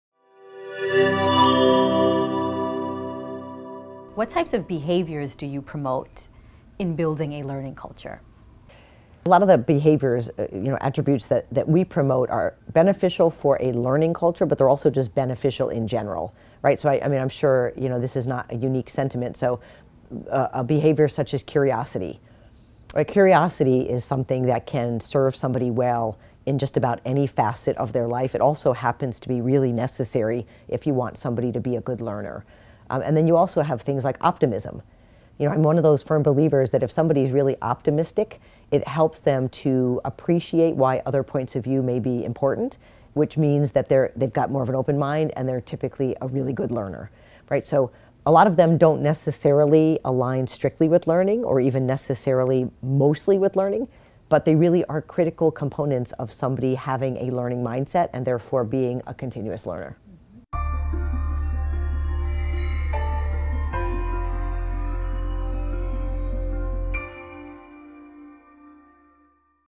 How do you promote learning behaviors? by Karen Kocher, CLO at Cigna
Karen is employed by Cigna as the Chief Learning Officer for the global organization.  In this role Karen is accountable for Learning, Leadership Effectiveness and Organizational Development for the entire Cigna team and also external stakeholders.  Specifically, Karen is responsible for capability and competency development, strategic workforce planning, employee engagement and the redesign of Cigna’s performance management model.
Prior to joining Cigna, Karen worked at IBM in multiple roles including as a Global Solutions Executive in the Software Division and an Offering Executive for Advanced and Emerging Technologies in Global Services.  Prior to IBM Karen was the General Manager/VP and P&L leader for the IKON Office Solution’s New England Learning Services business.
The early years of Karen’s career were spent in line positions in insurance and financial services including finance, operations, information technology, risk management and sales.  Karen earned the prestigious Chartered Property Casualty Underwriter (CPCU) designation from the American College and is a member of the CPCU Society.

Karen Kocher talks about the different attributes such as optimism, having an open mind, and being curious, that are encouraged at Cigna, being both beneficial to the individual and for learning.